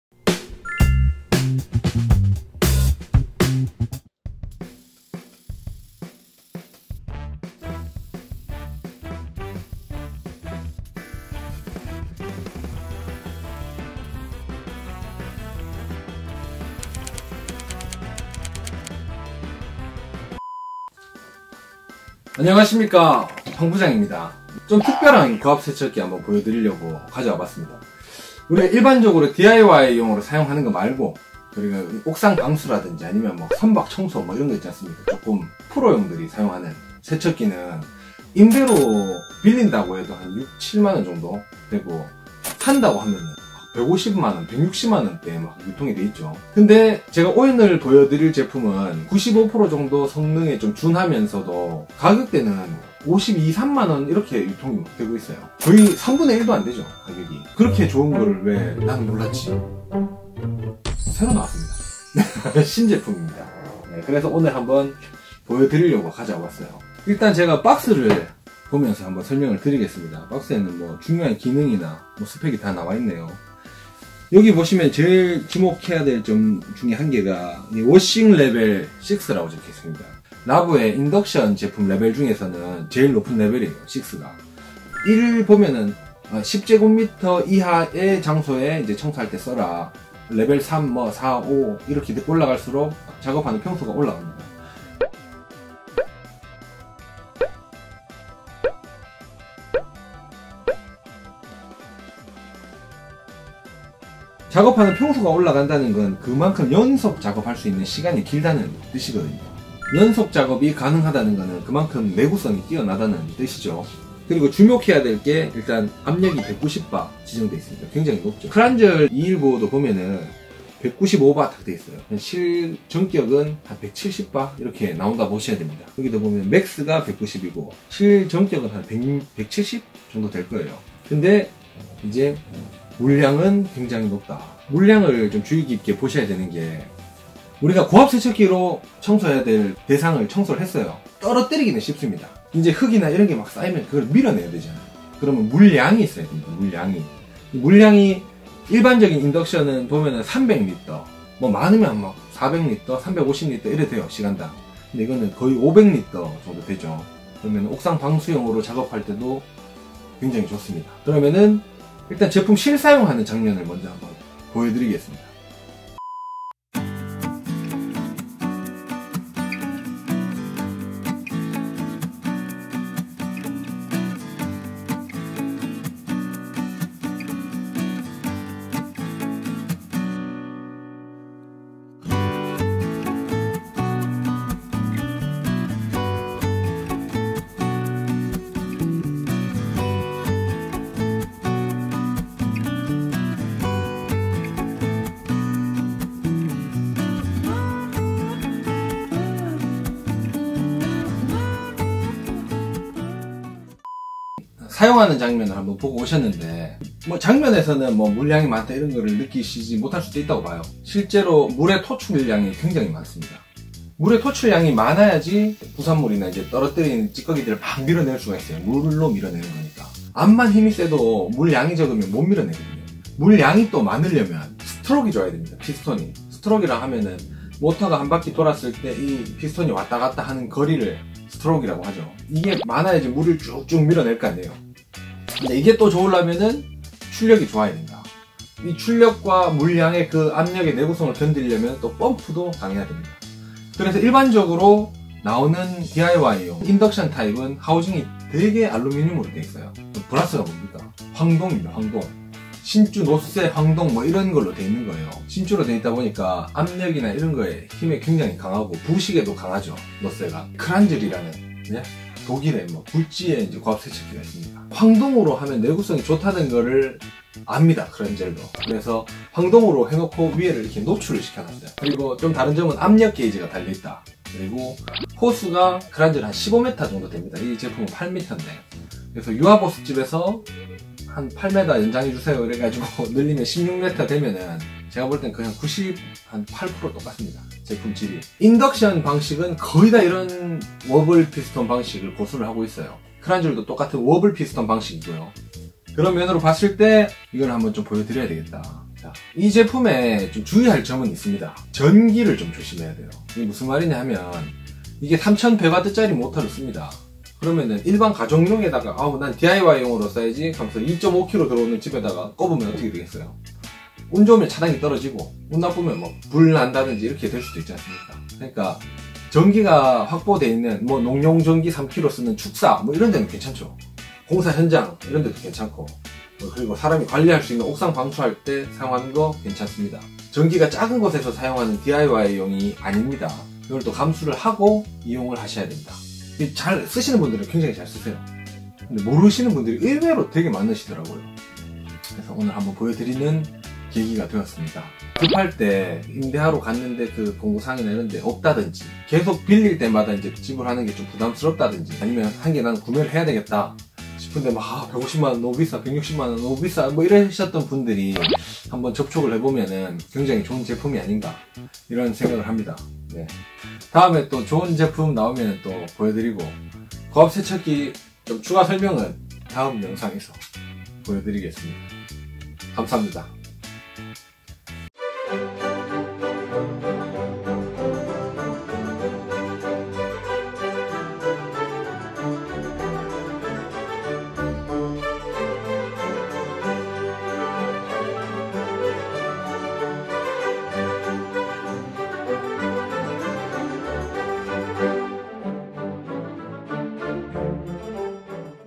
옥상방수!전문가용 고압세척기 105만원 싸게 사는방법!? 라보 브라스 310(LAVOR BRASS 310 High pressure water blaster)
제품 구매처 링크입니다
툴스24 공구마트